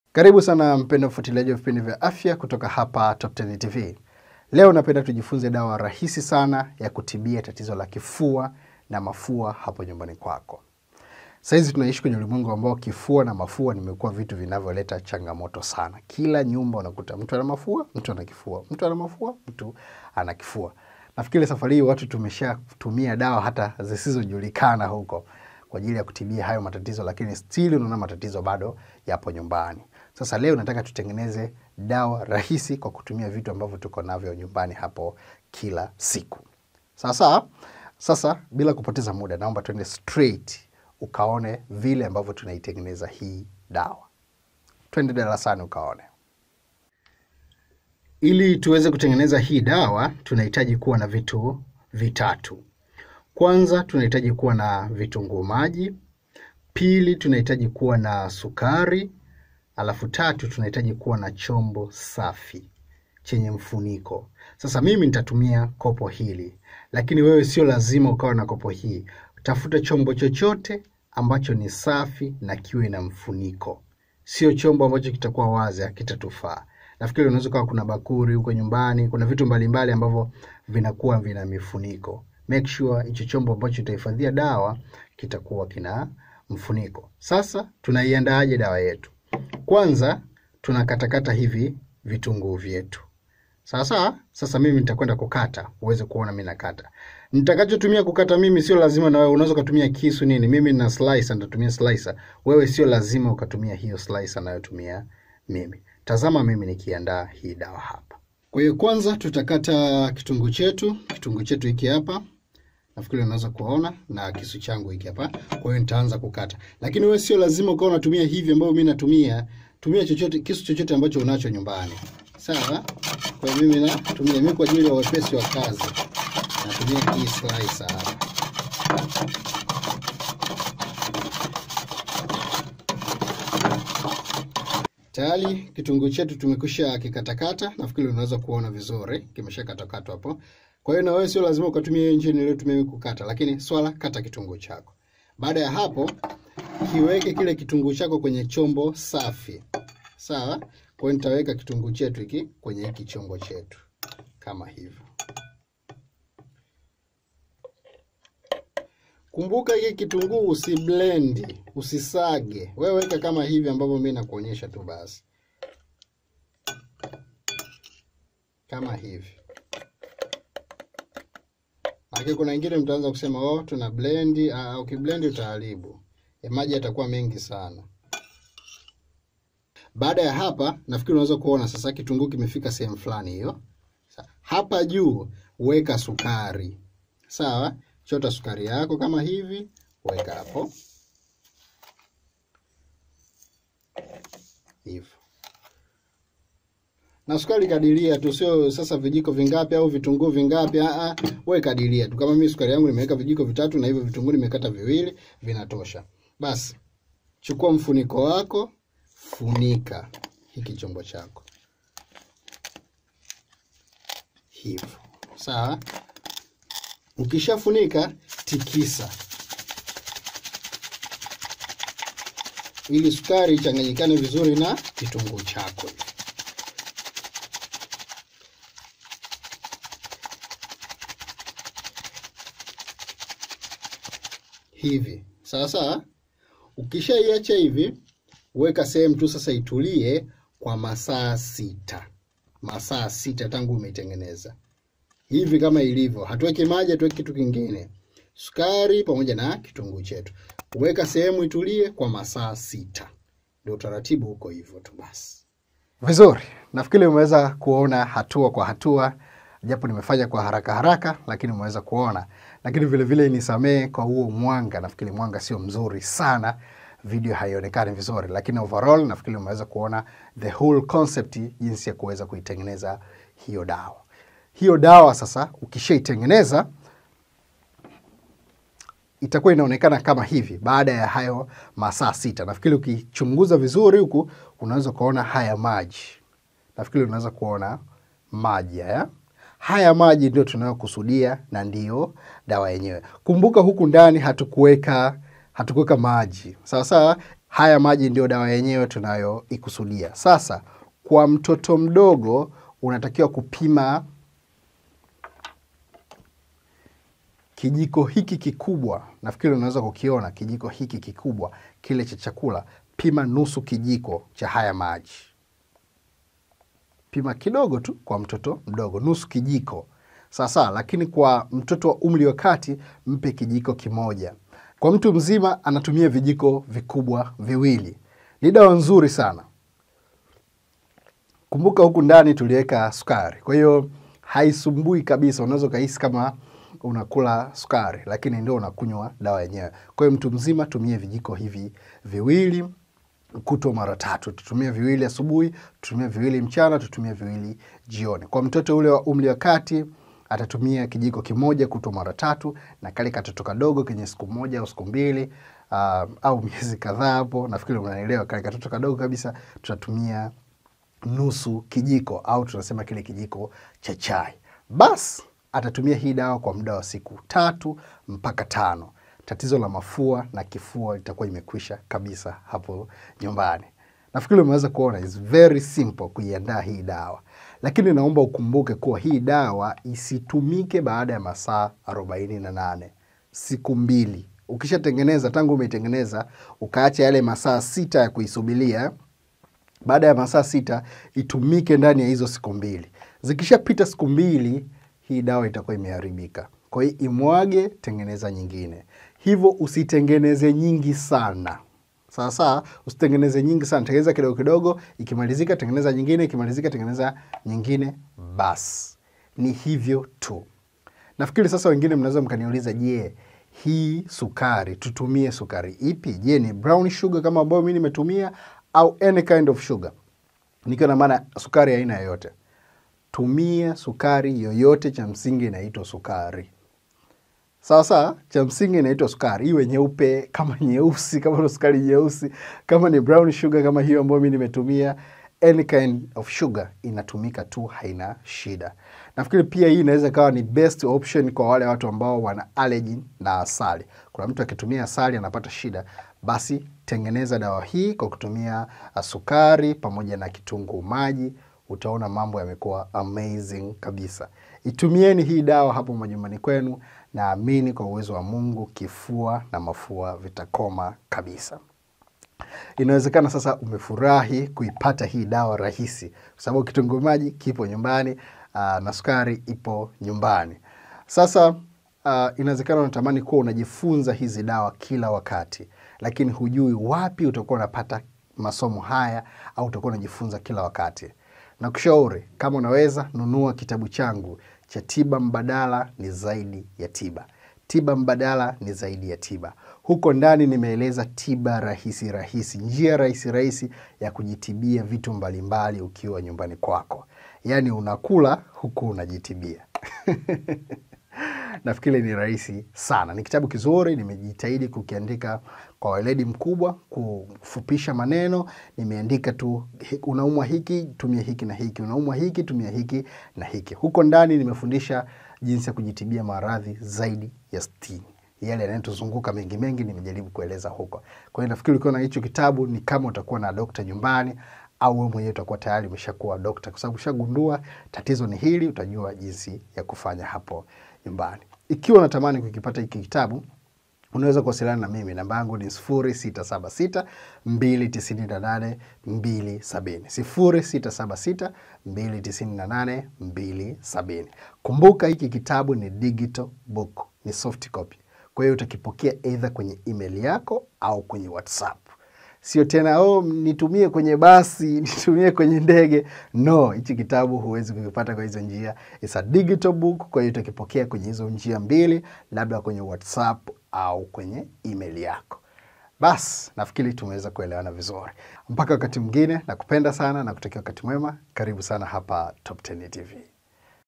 Maajabu Ya kitunguu katika Kutibu kifua
Dawa hii ni nzuri sana na inafanya kazi kwa haraka sana... Zingatia tu jinsi ya kuiandaa na kuitumia kama nilivyoelekeza hapa